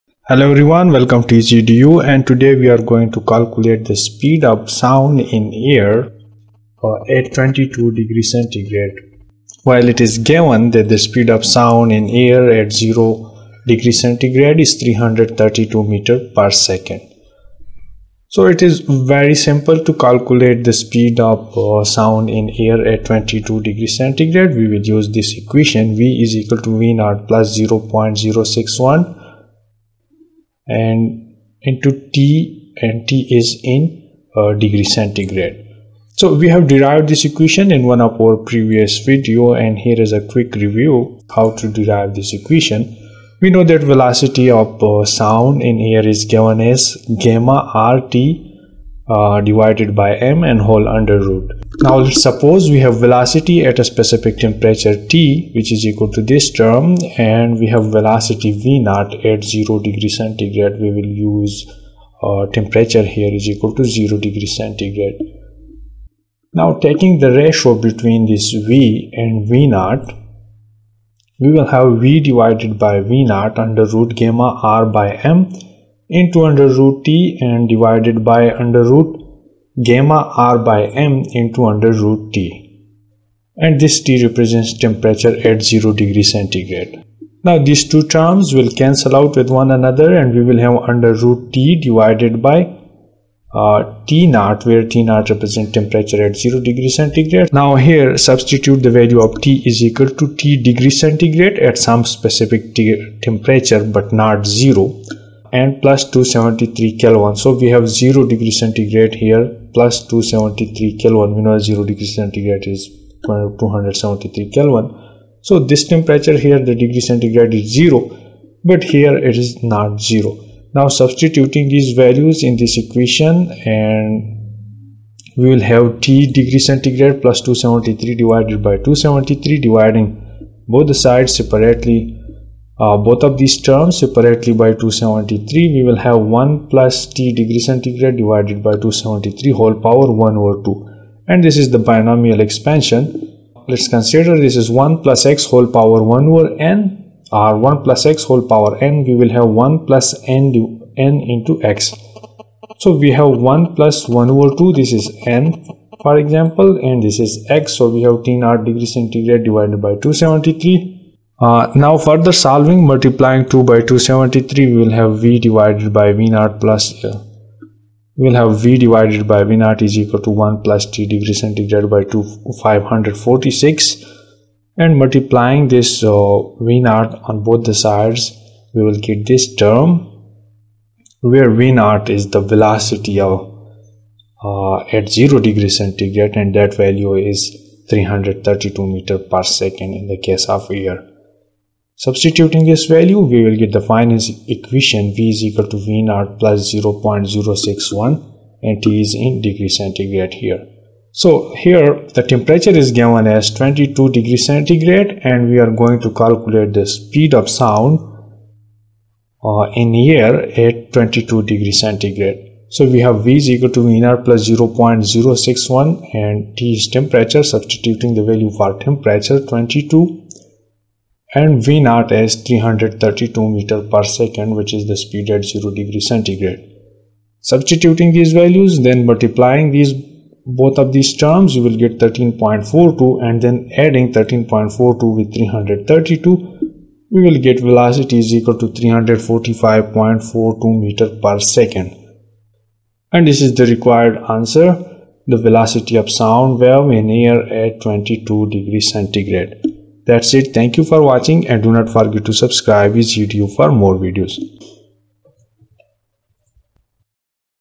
How to Calculate the Speed of Sound at 22°C in air
The speed of sound in air changes with temperature. At 0°C, the speed of sound is known to be 332 meters per second (m/s). To calculate the speed of sound at any other temperature, such as 22°C, we use a formula that relates the speed of sound to temperature. This formula is based on the principle that sound travels faster in warmer air due to the increased energy and movement of air molecules.

Here’s how we calculate it:

We start with the general formula:

​v_t=v_0 underoot (T+273) / 273

 v_0 is the speed of sound at 0°C, which is 332 meters per second.
𝑇
T is the temperature in Celsius.
For this example, we want to find the speed of sound at 22°C:

First, add 273 to the temperature in Celsius (22°C), which gives you 295 Kelvin.
Divide this by 273 Kelvin, which gives you a ratio of approximately 1.0802.
Find the square root of this ratio, which is approximately 1.039.
Multiply the original speed of sound at 0°C (which is 332 m/s) by this square root. So, 332 m/s × 1.039 equals approximately 345 meters per second.
Thus, the speed of sound at 22°C is about 345 meters per second, showing that sound travels faster in warmer air. This increase in speed happens because the warmer temperature increases the energy and movement of air molecules, making it easier for sound waves to propagate.